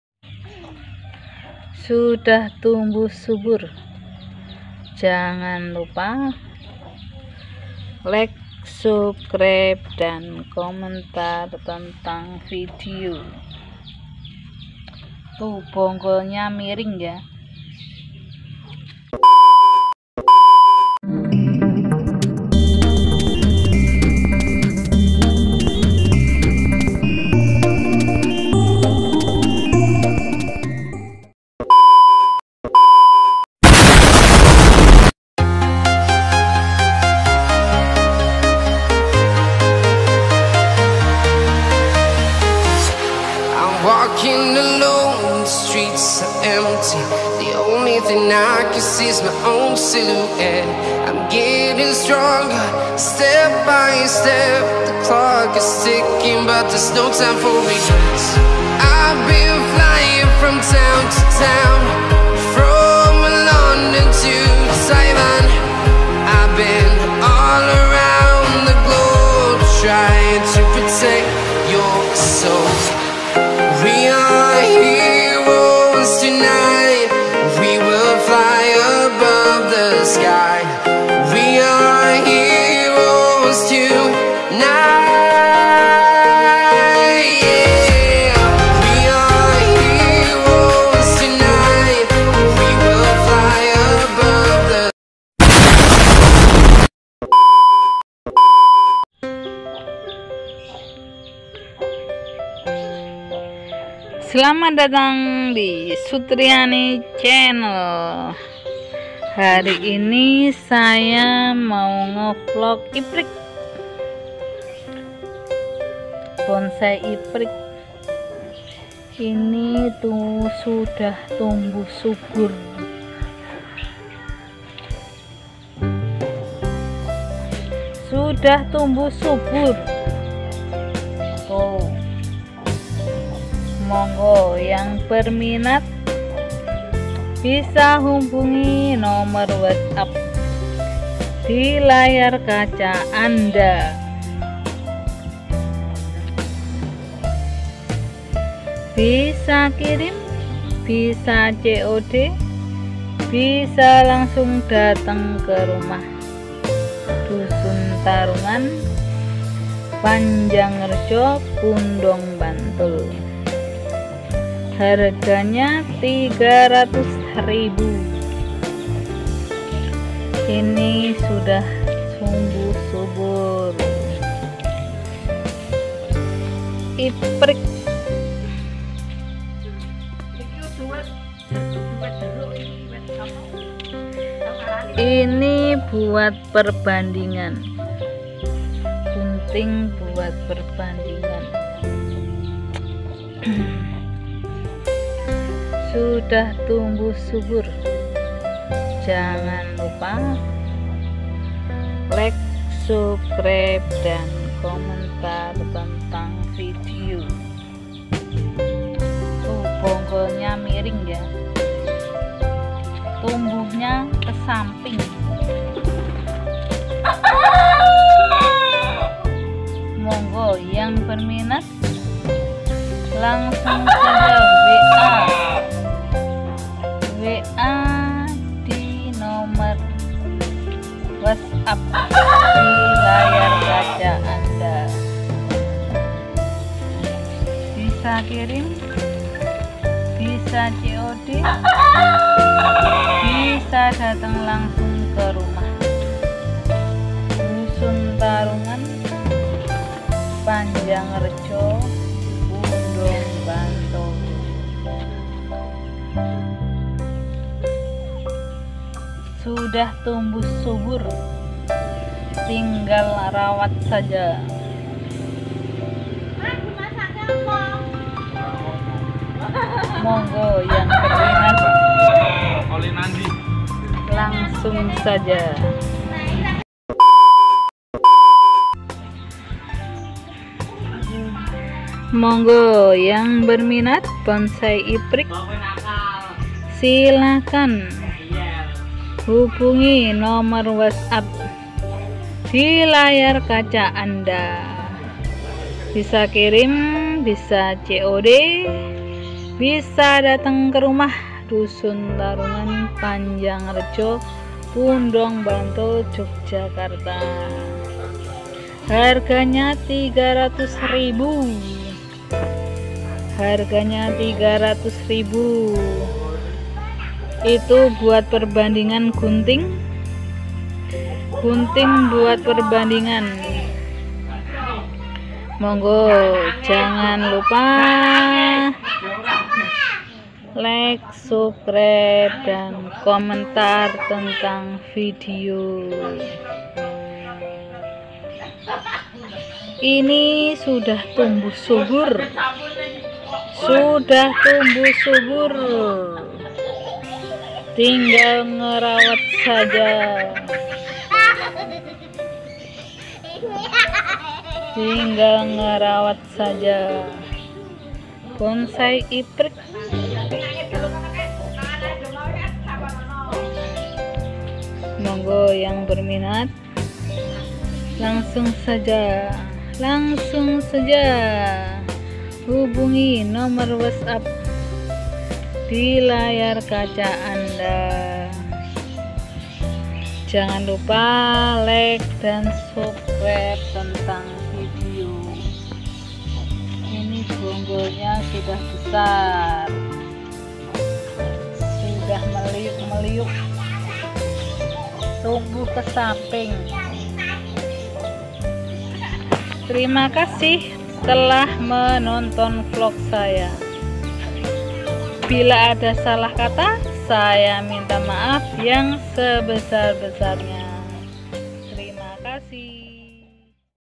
bonsai iprik bagus cuma 300 ribu Pundong Bantul Jogjakarta
bagi pecinta bonsai silahkan di beli Bonsai iprik subur makmur harga 300 ribu Sangat bagus tinggal di bentuk aja
lokasi Pundong Bantul Jogjakarta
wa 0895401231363